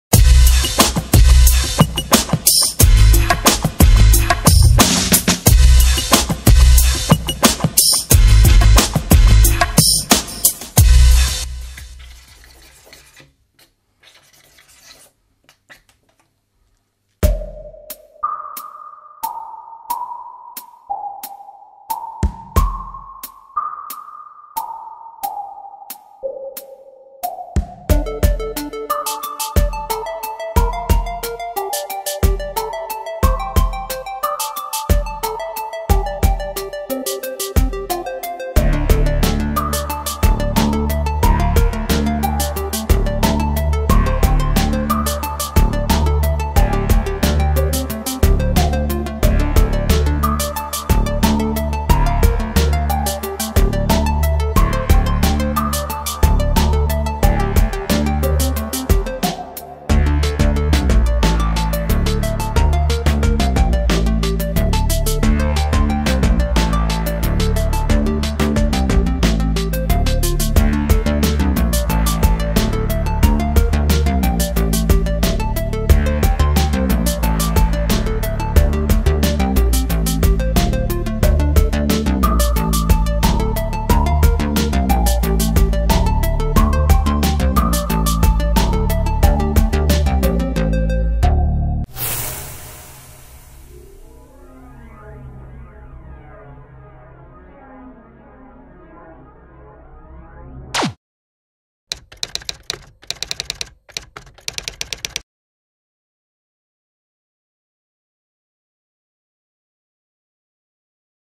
Saikone: Graffiti Tutorial #041 "Style-Alphabet" # U #
In dieser Folge zeigen wir euch den Buchstaben "U". Wie immer in Blockstyle, Bubblestyle und Wildstyle. Viel Spaß :)!
Vielen Dank für die Beatz an Stephan Günther!